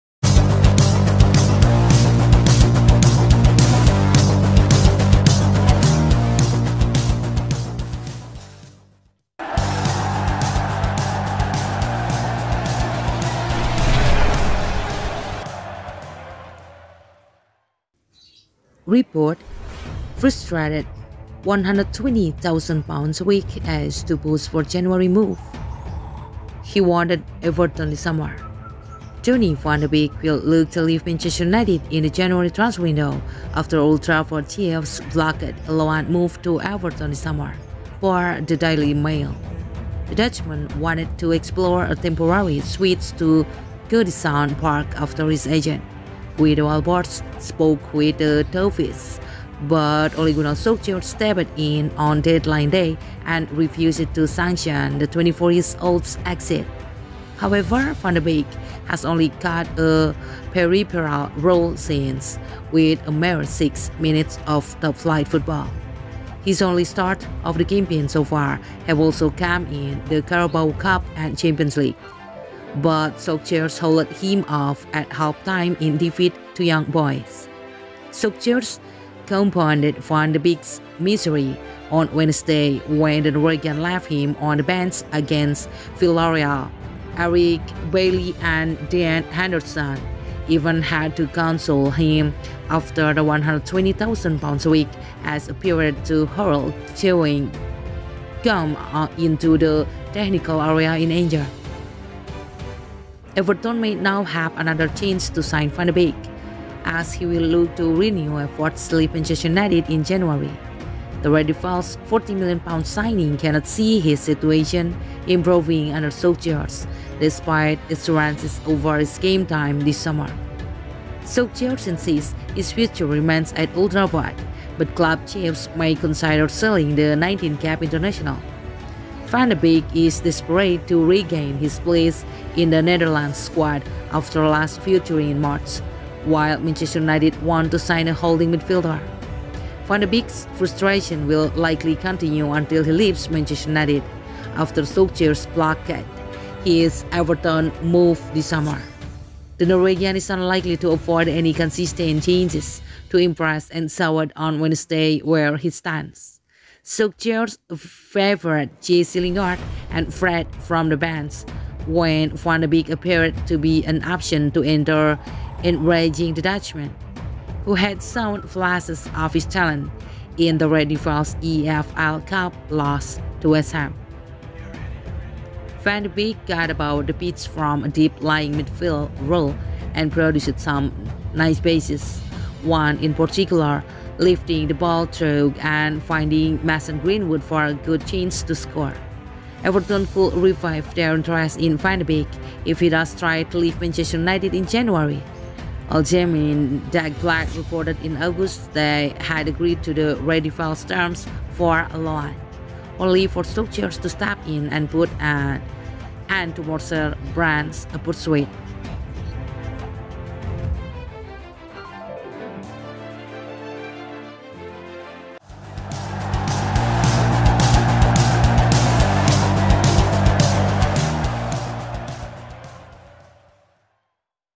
Sold Report: Frustrated £120k-a-week ace to push for January move, he wanted Everton this summer
Donny van de Beek will look to leave Manchester United in the January transfer window after Old Trafford chiefs blocked a loan move to Everton this summer, per the Daily Mail.

The Dutchman wanted to explore a temporary switch to Goodison Park after his agent, Guido Albers, spoke with the Toffees. But Ole Gunnar Solskjaer stepped in on Deadline Day and refused to sanction the 24-year-old’s exit.

However, Van de Beek has only cut a peripheral role since, with a mere six minutes of top-flight football. His only starts of the campaign so far have also come in the Carabao Cup and Champions League. But Solskjaer hauled him off at half-time in defeat to Young Boys.

Solskjaer compounded Van de Beek’s misery on Wednesday when the Norwegian left him on the bench against Villarreal. Eric Bailly and Dean Henderson even had to console him after the £120k-a-week ace appeared to hurl chewing gum into the technical area in anger.

Everton may now have another chance to sign Van de Beek, as he will look to renew efforts to leave Manchester United in January. The Red Devils’ £40m signing cannot see his situation improving under Solskjaer, despite assurances over his game time this summer.